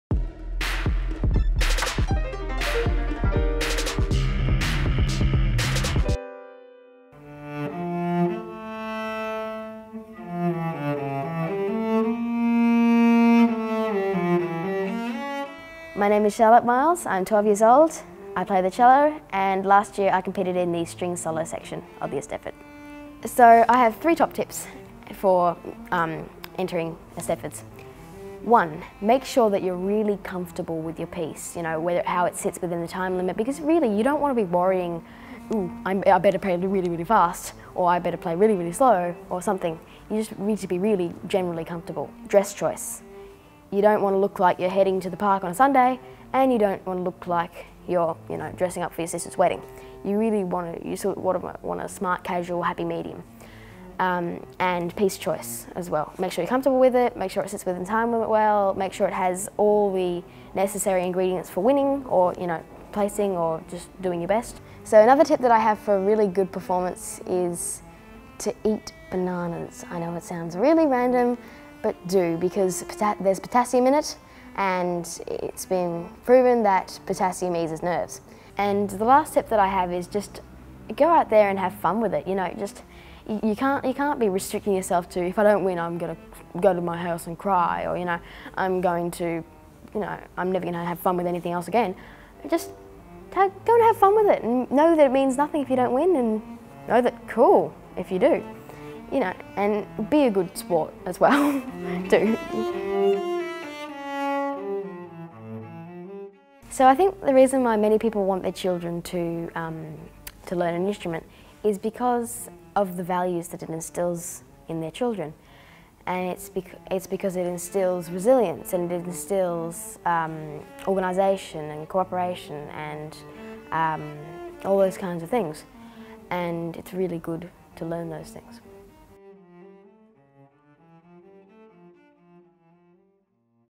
Introducing Cello Player Charlotte Miles
Charlotte gives us her 3 top tips for the Eisteddfod, based on her experience as an accomplished musician. She talks about the values that music education instills in young people.

Video by Scott Alexander